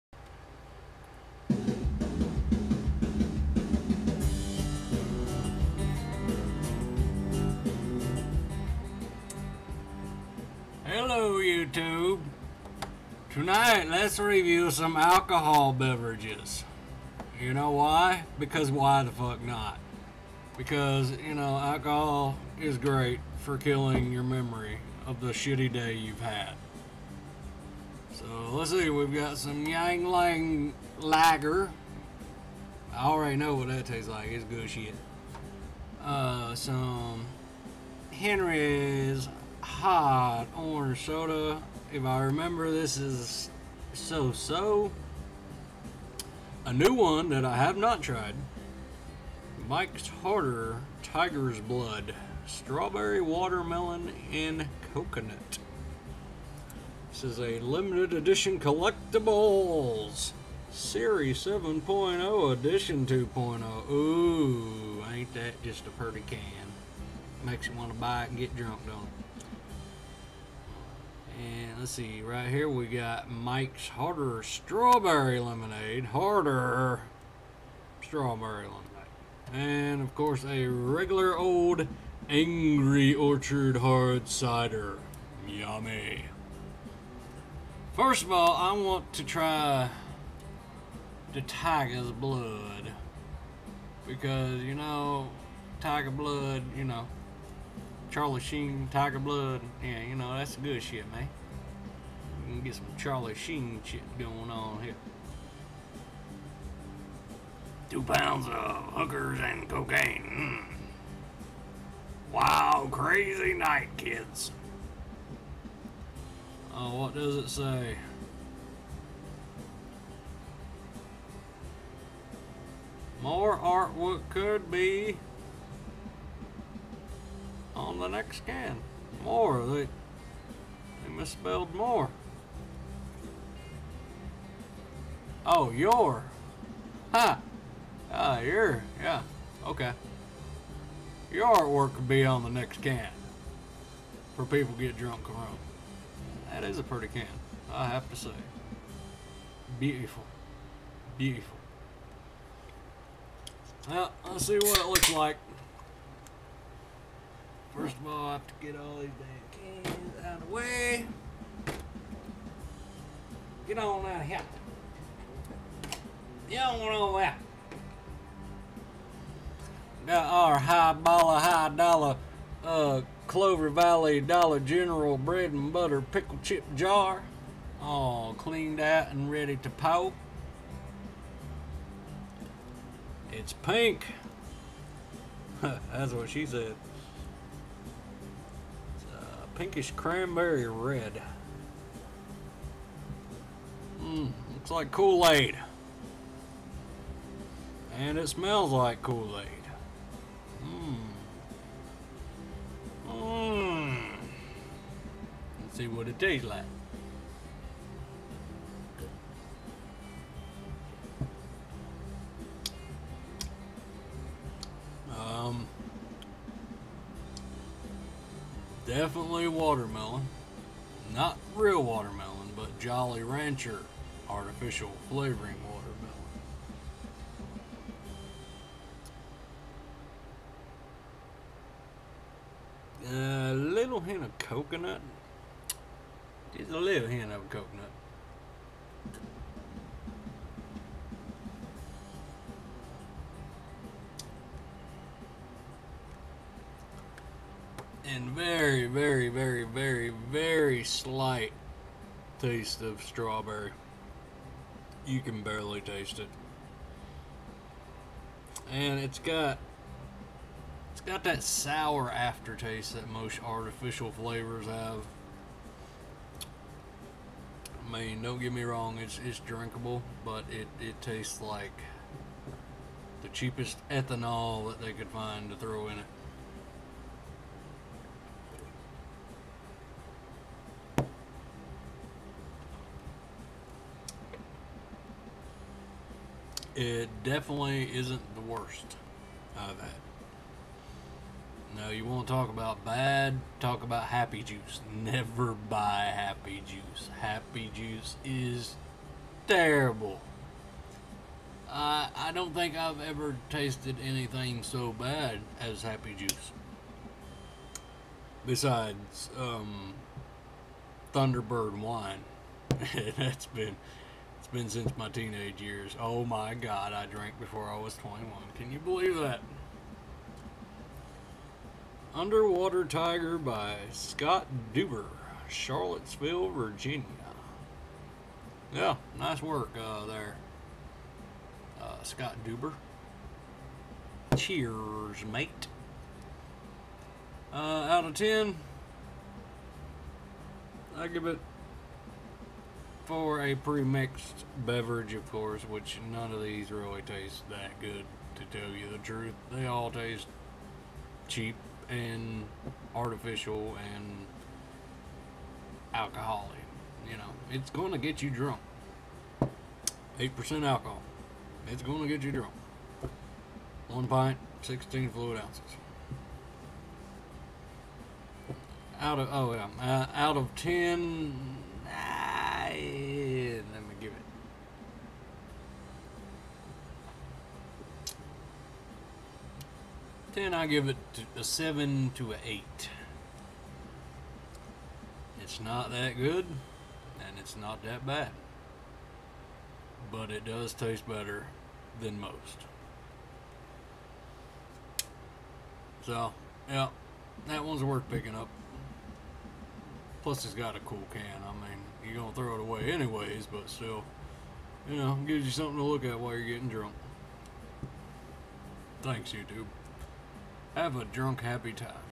Mike's Harder: Tiger's Blood Review
Mike's Harder: Tiger's Blood
It tastes alright, it's in a groovy can, and it'll get ya drunk for bout 2 dollah n' some change.
8% Alcohol 16fl oz
 7/10